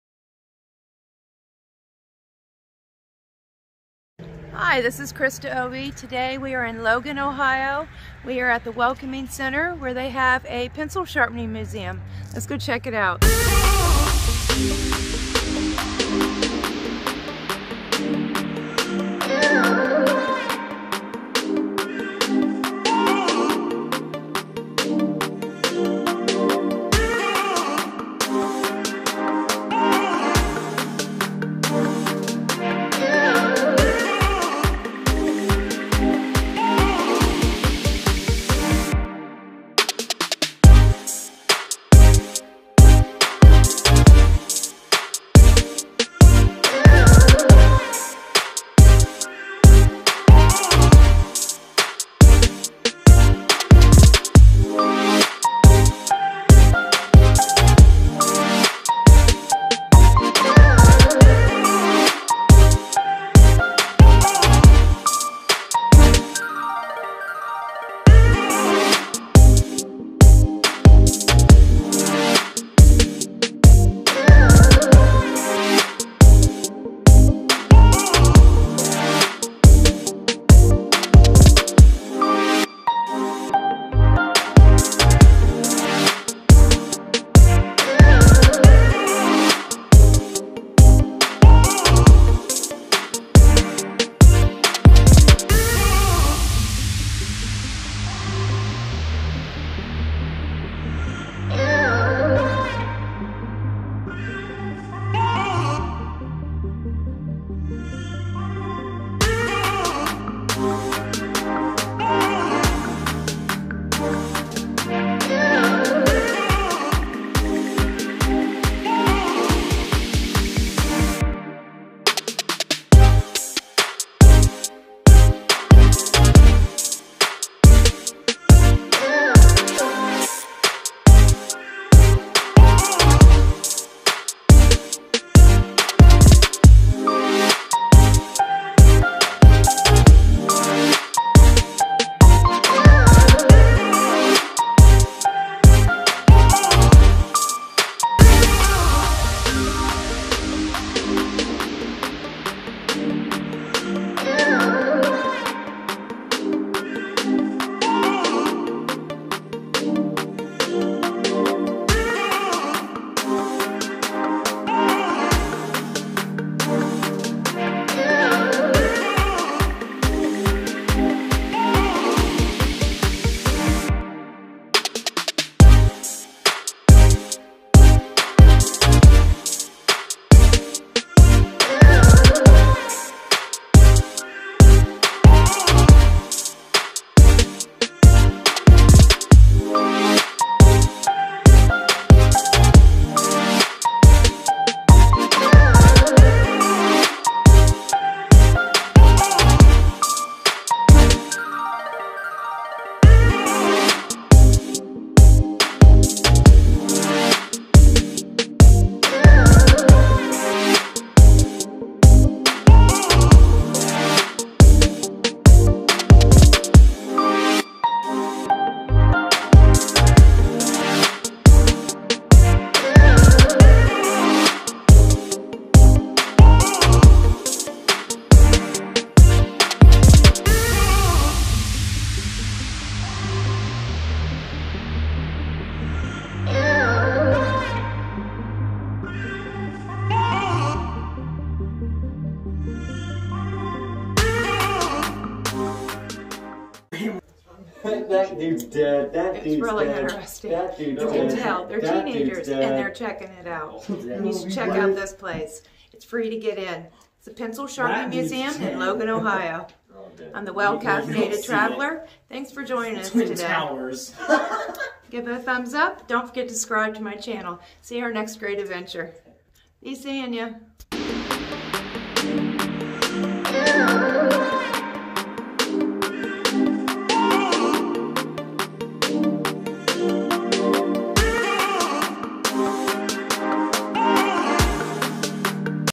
Hocking Hills || Pencil Sharpener Museum
If you are in the Hocking Hills area, then by all means make a stop at this unusual museum. The collection is housed in a small shed like building by the Visitors Center. If you grew up using hand held pencil sharpeners, then you'll get a kick out of this collection.